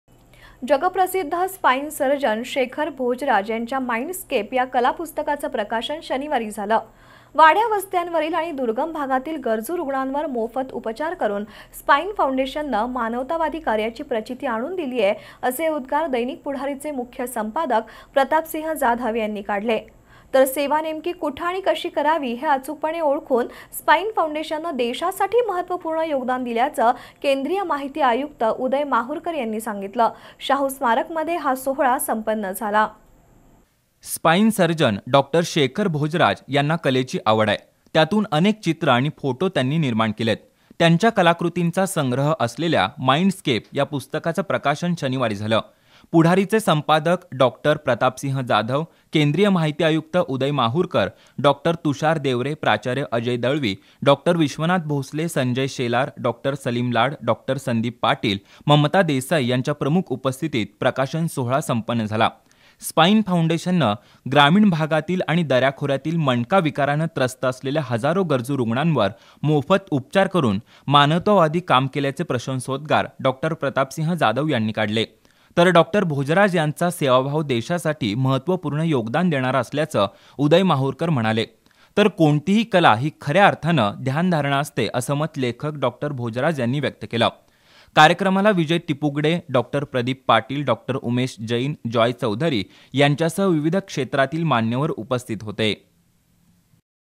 जगप्रसिध्द स्पाईन सर्जरी शेखर भोजराज यांच्या माईर्ंडस्केप या कलापुस्तकाचं प्रकाशन संपन्न
जगप्रसिध्द स्पाईन सर्जरी शेखर भोजराज यांच्या माईर्ंडस्केप या कलापुस्तकाचं प्रकाशन विविध मान्यवरांच्या उपस्थितीत संपन्न
Channel B